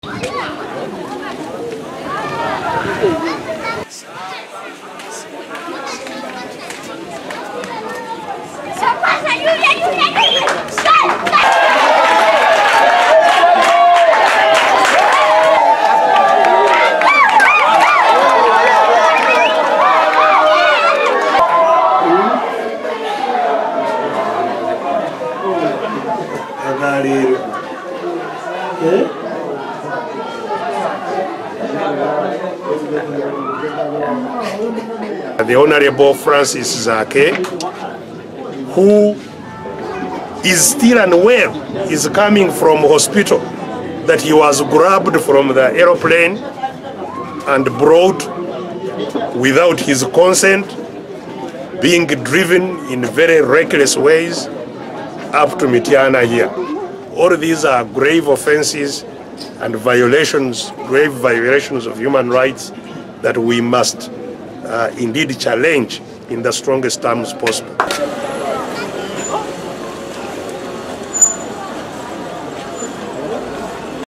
Besigye visits Zaake, condemns security forces
The home of Mityana Municipality MP Francis Zaake is now a bee-hive of activity as visitors pour in to welcome him back home from India where he had been taken for treatment of injuries sustained in the Arua fracas.

Former FDC party president Dr. Kiiza Besigye, also made his way to Mityana where he meet with the ailing MP, Zaake. Dr. Besigye appreciated the treatment offered to Francis Zaake while in India but condemned the way the MP was handled on arrival at Entebbe airport.